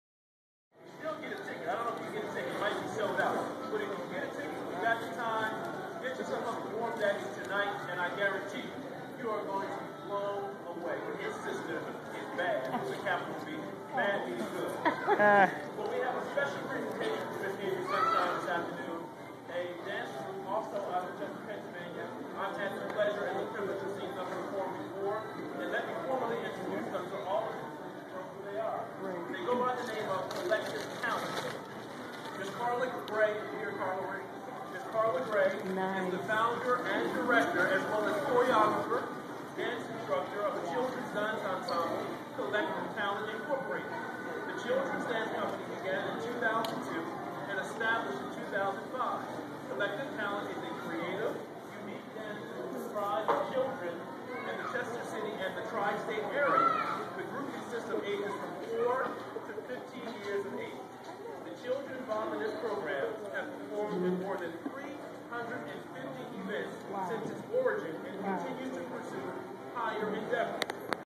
Kollective Talent 2011-10-15_14
Dancing for Avery Sunshine at Tristate Mall Claymont, DE